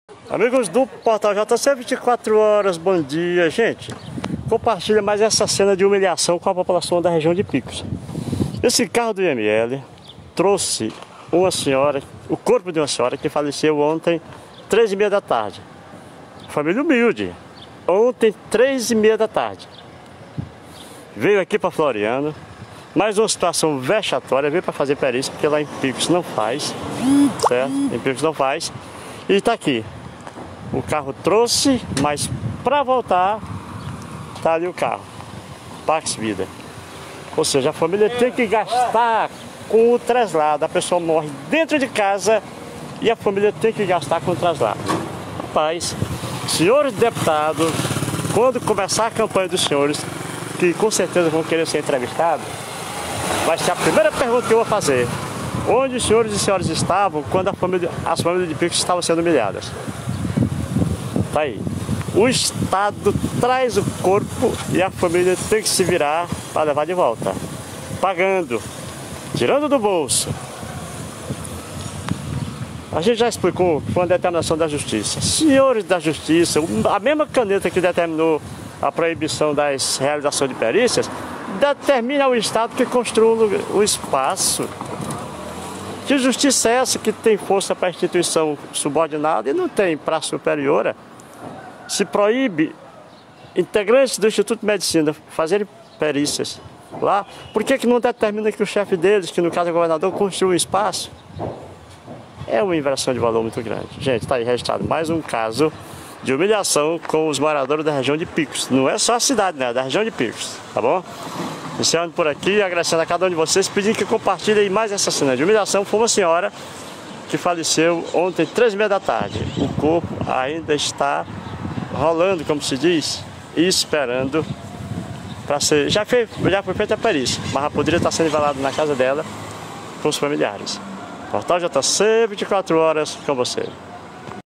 Mulher que faleceu ontem(28) a tarde em Picos teve o corpo periciado hoje em Floriano
O Portal jc24horas registrou mais um caso de humilhação para com a população de Picos. Uma senhora cujo nome não foi revelado,  faleceu ontem naquela cidade por volta das 15h30min e o seu corpo teve que ser encaminhado para ser submetido a exames periciais em Floriano, tudo isso porque a justiça do trabalho proibiu que esse procedimento fosse realizado naquela cidade, da mesma maneira que é realizado em Floriano.
A mesma justiça que proibiu a ação da perícia deveria obrigar o governo a construir um lugar para que os familiares das pessoas vítimas de mortes violentas não sejam mais submetidas a humilhações como esta.